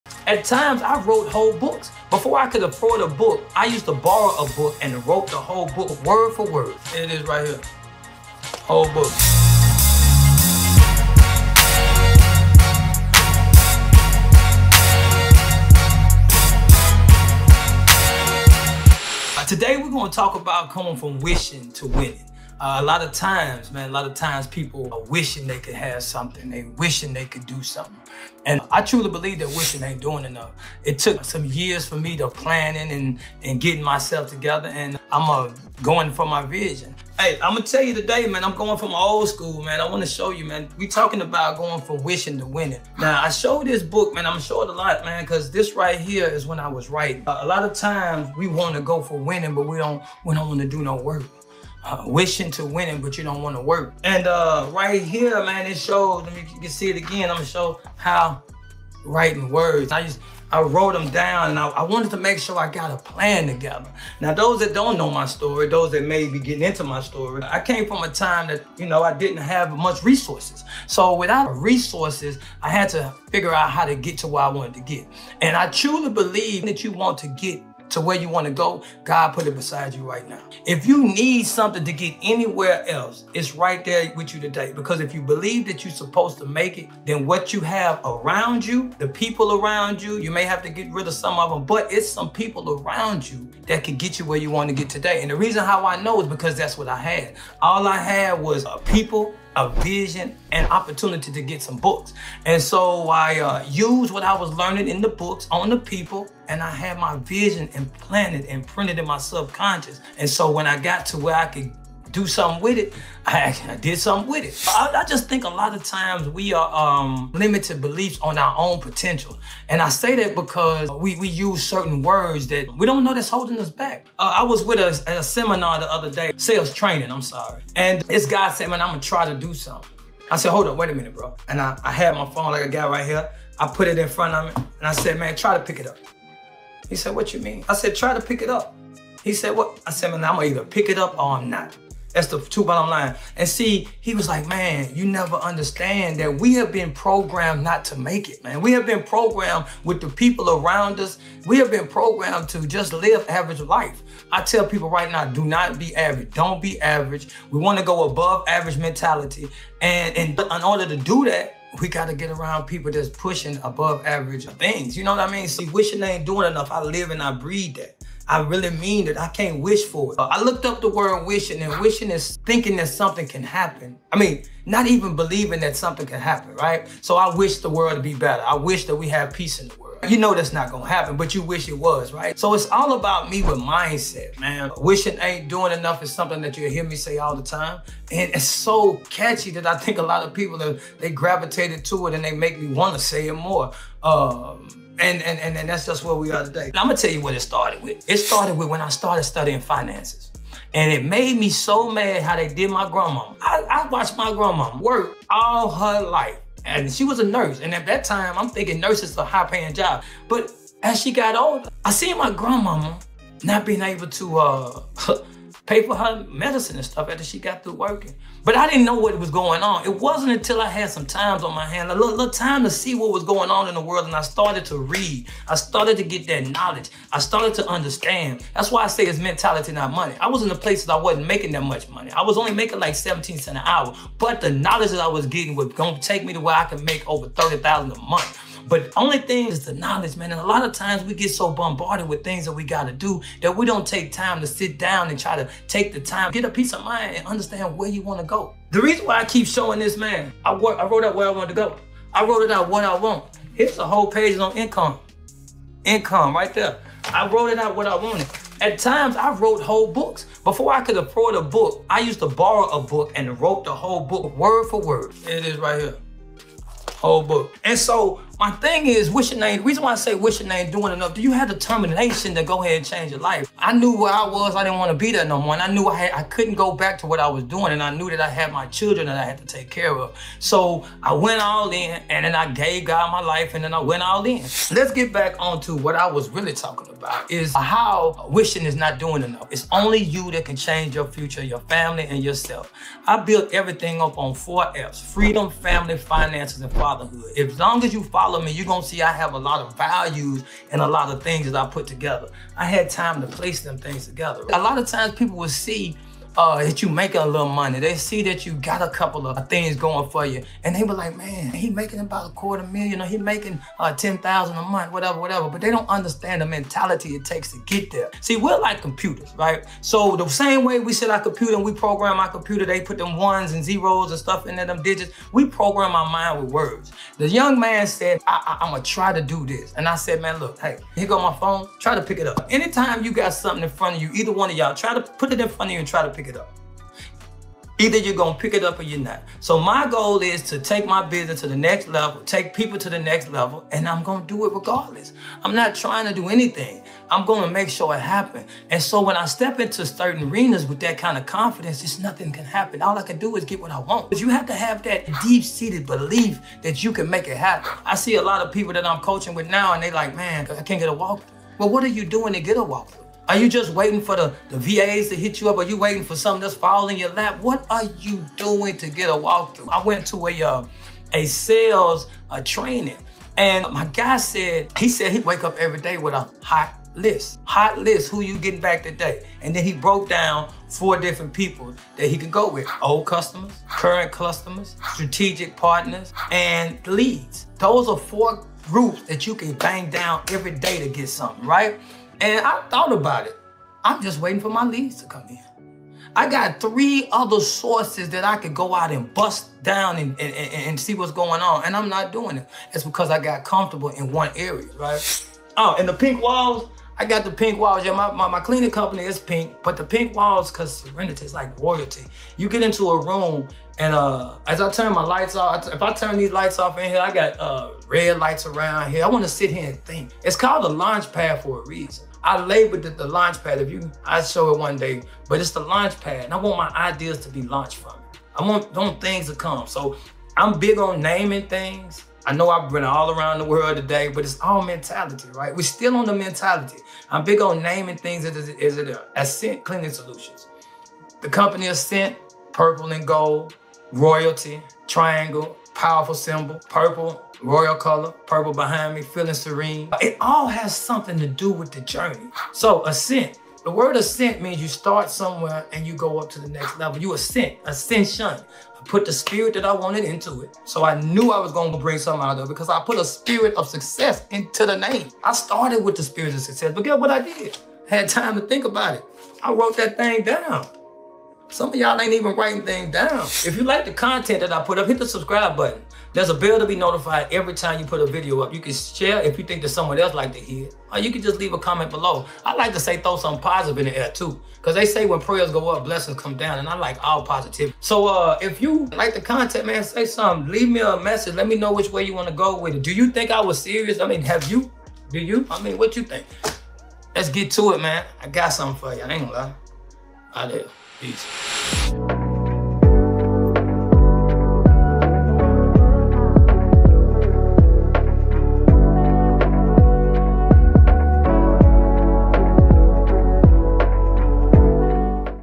Wishing to Winning: The Unconventional Path to Success
🚀 Ready for a transformative journey? In our latest video, I share the untold story of going from wishes to real victories. Talks about how I wrote entire books before affording any of my own  and the mindset shift that led to building multiple businesses

🌟 Discover the keys to success, breaking free from limited beliefs, and turning aspirations into tangible achievements.

📚 From humble beginnings to financial freedom, this video is a guide for those ready to transform their wishes into triumphant wins. Dive into the importance of self-programming, utilizing knowledge, and crafting a vision that propels you towards your goals, even when resources are scarce.

🔑 This motivational session challenges you to break free from average thinking, redefine your goals, and surround yourself with positive influences. It's not just about making money; it's about the mindset that paves the way for extraordinary success.

👊 Ready to transform your wishes. Watch now for a dose of inspiration!

💡 Let's build a community of achievers – because your success story starts with the right mindset! 🌟💰